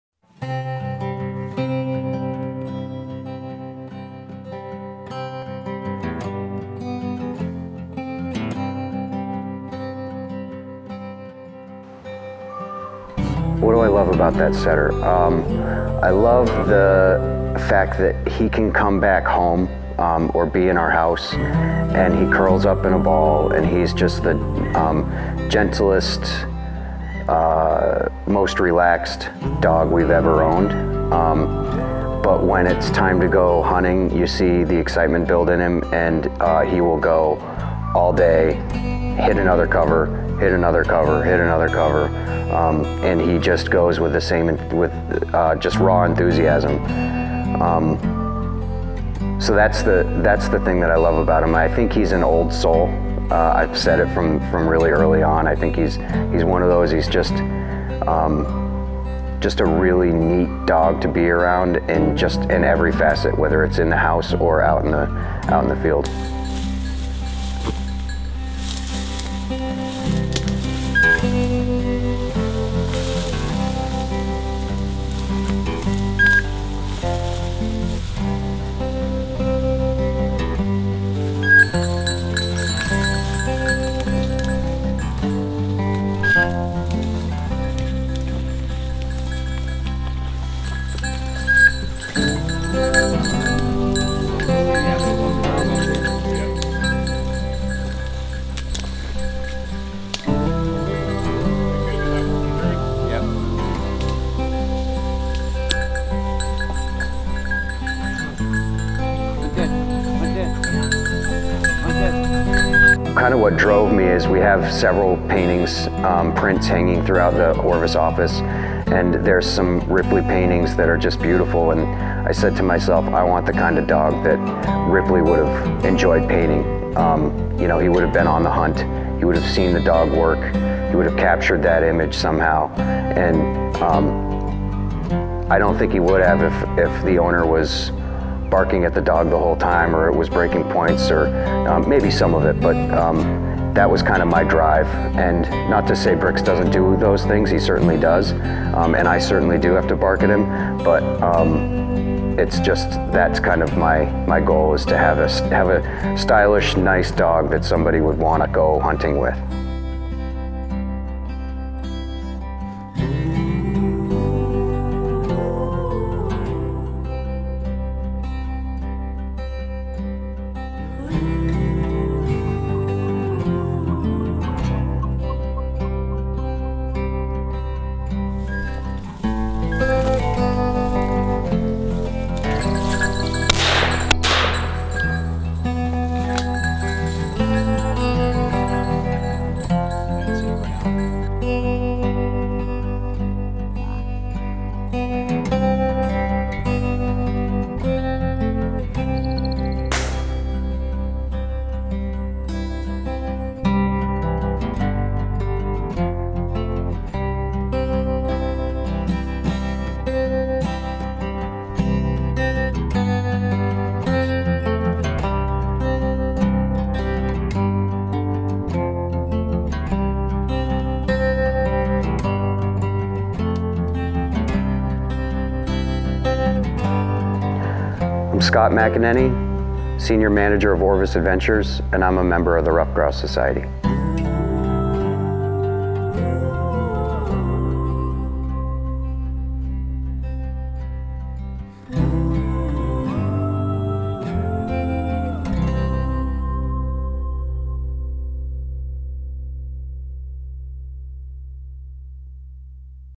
Scott McEnaney of Orvis // HealthyForests // Ruffed Grouse Society
Scott McEnaney is a Vermont native that takes New England culture to the next level. When you do not find him tapping Maple trees for syrup or at the office for Orvis. You can find him leading the charge in a local chapter for the Ruffed Grouse Society to help promote healthy forests in the area not just for himself but for future generations.